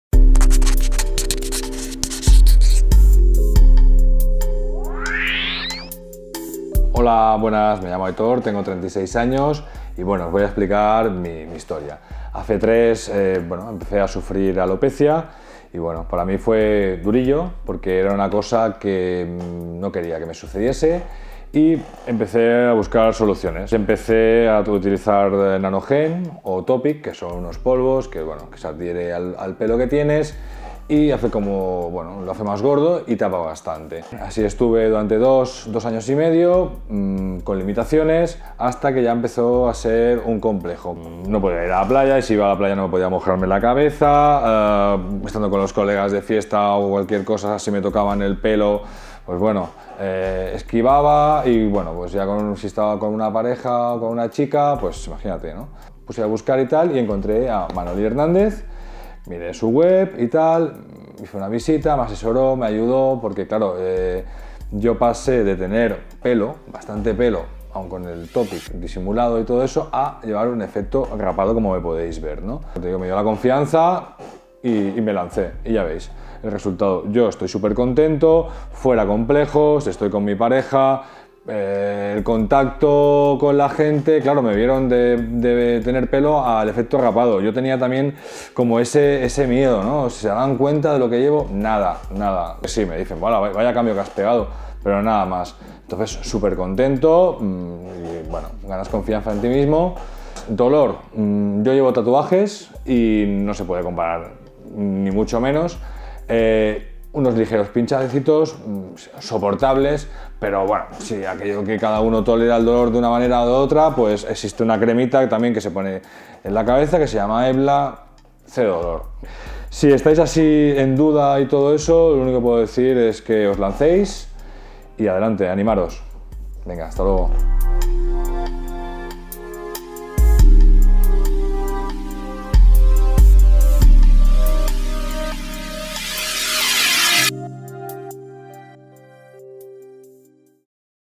Aitor:Testimonio de micropigmentación capilar - Manoli Hernández
Testimonio de Aitor, un chico de Barcelona que se ha realizado un tratamiento de micropigmentación capilar en nuestro centro.